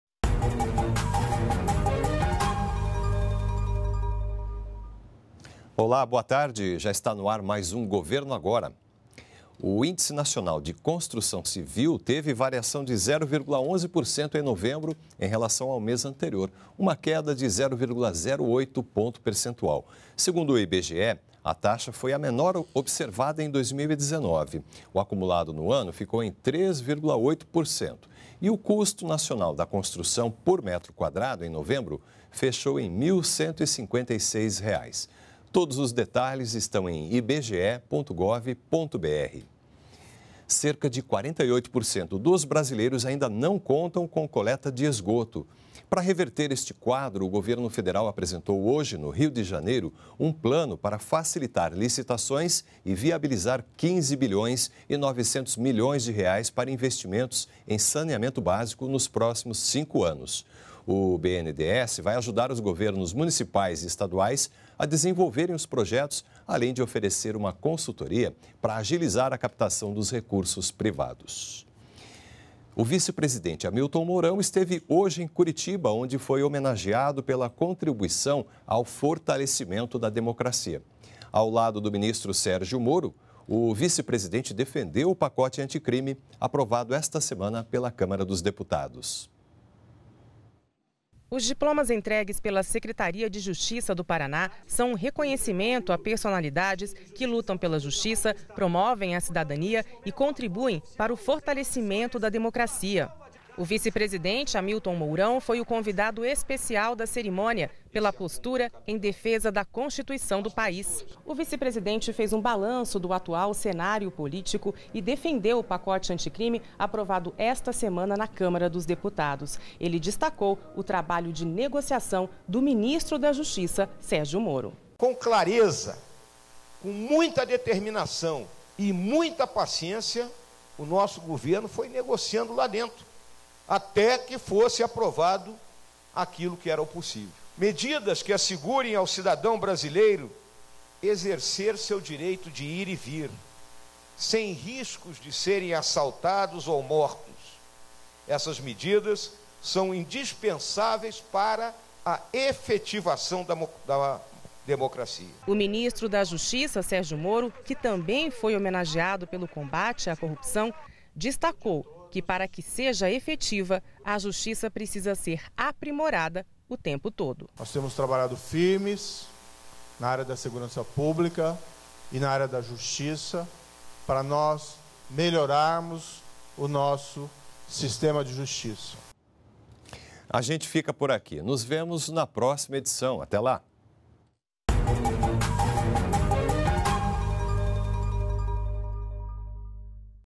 Hamilton Mourão homenageado por fortalecer a democracia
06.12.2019 - O Índice Nacional de Construção Civil teve variação de 0,11% em novembro, em relação a outubro. Governo federal apresentou um plano para viabilizar R$ 15,9 bilhões investimentos em saneamento básico. E o vice-presidente Hamilton Mourão esteve hoje em Curitiba (PR), onde foi homenageado  pela contribuição ao fortalecimento da democracia.